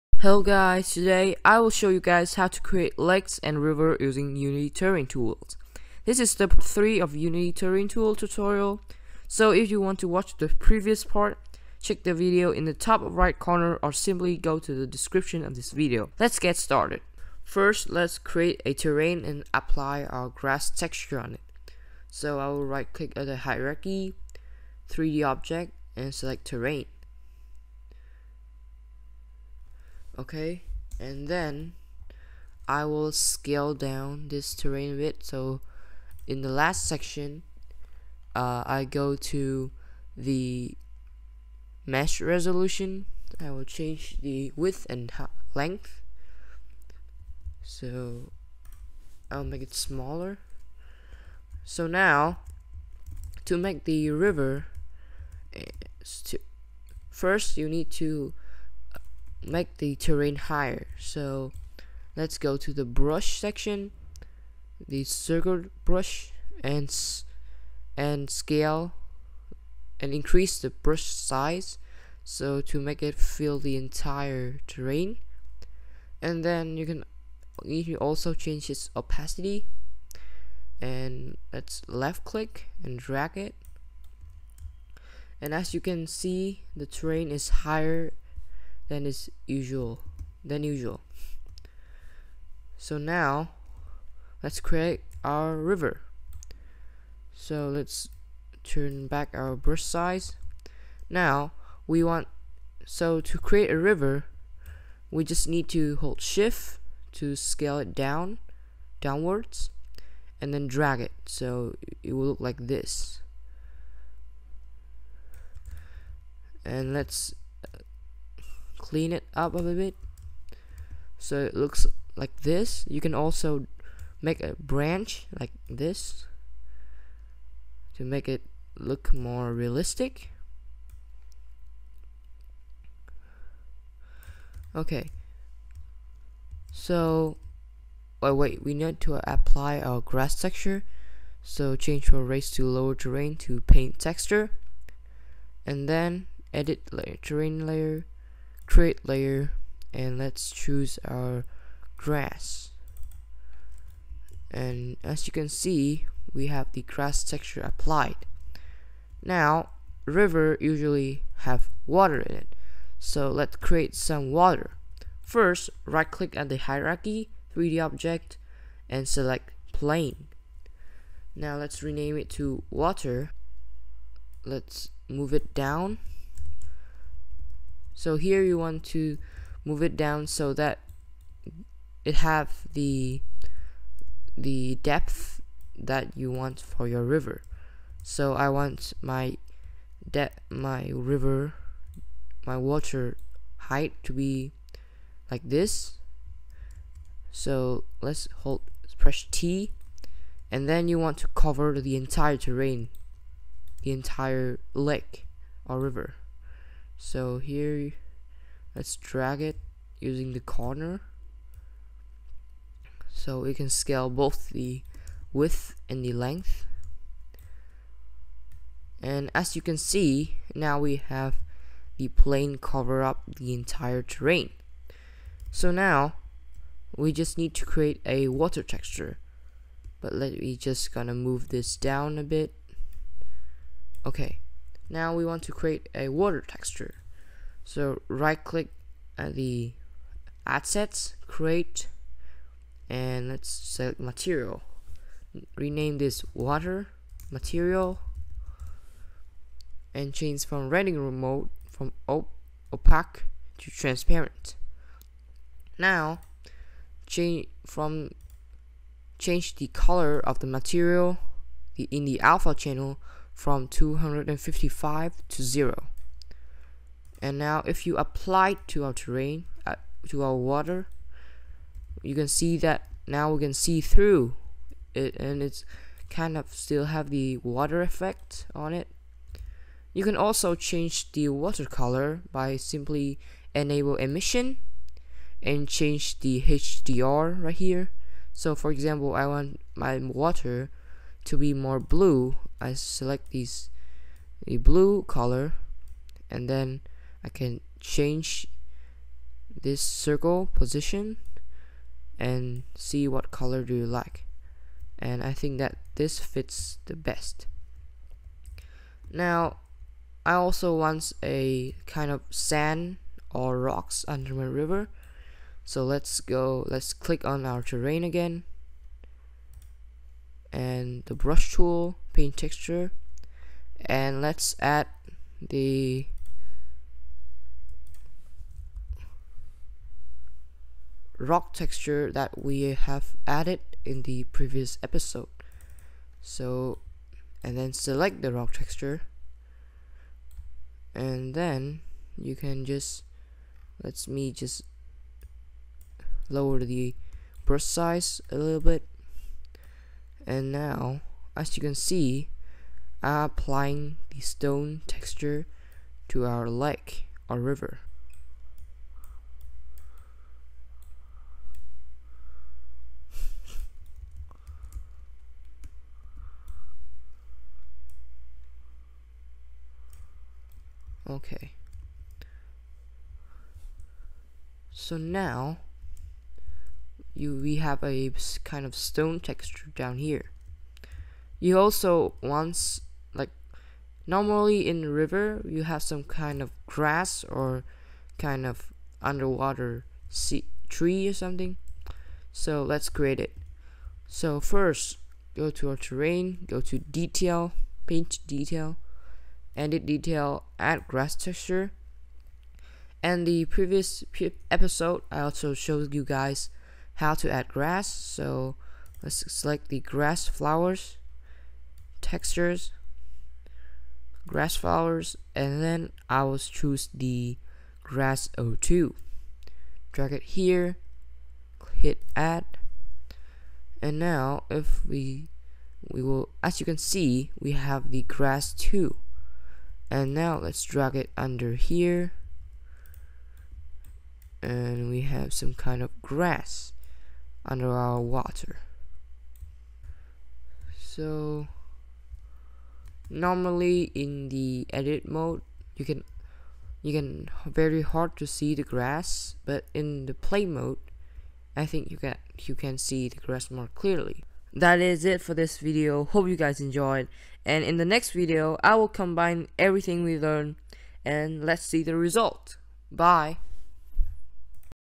How to Creates River and Lakes to your Terrain in Unity! Ep#3
In this video, I will show you guys how to add rivers and lakes to your terrain. This makes your game terrain much more natural! Make sure to check out the other episode as well!

Timestamps🕐:
0:00